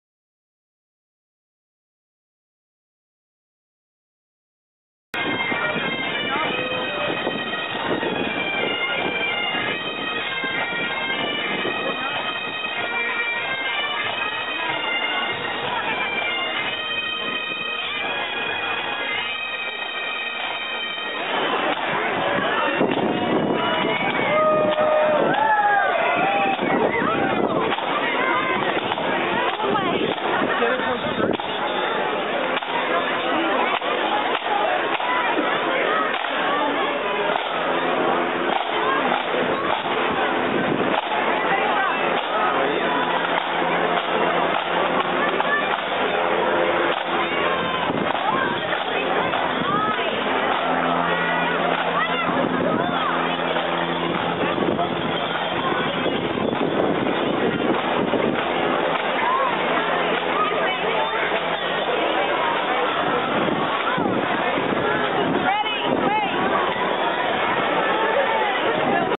Santa Claus Parade Toronto 2010 part 2
One of the most amazing events for all citizens of Toronto. Lots of impressions and fun for little ones and their parents.